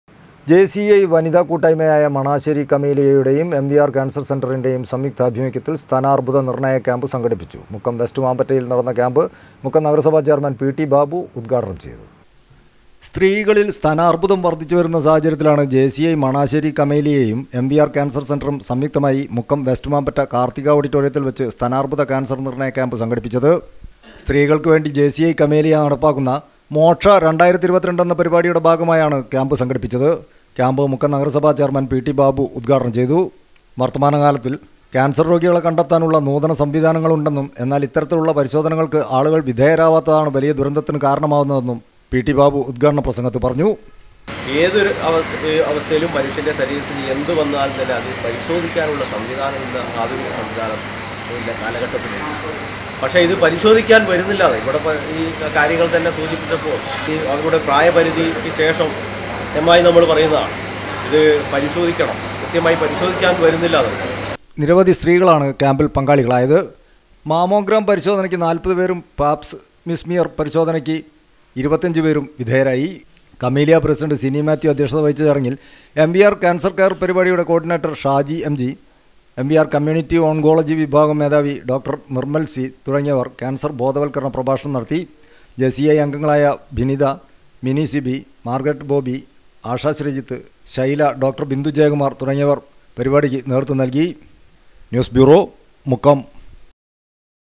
സ്തനാർബുദ നിർണ്ണയ ക്യാമ്പ് സംഘടിപ്പിച്ചു
ജെ.സി.ഐ.യുടെ  വനിതാകൂട്ടായ്മയായ മണാശ്ശേരി  കമീലയയുടെയും എം.വി.ആർ .ക്യാൻസർ സെന്ററിന്റെയും സംയുക്താഭിമുഖ്യത്തിൽ സ്തനാർബുദ നിർണ്ണയ ക്യാമ്പ് സംഘടിപ്പിച്ചു  . മുക്കം വെസ്റ്റ് മാബറ്റയിൽ നടന്ന  ക്യാമ്പ്  മുക്കം നഗരസഭ ചെയർമാൻ പി ടി ബാബു ഉദ്ഘാടനം ചെയ്തു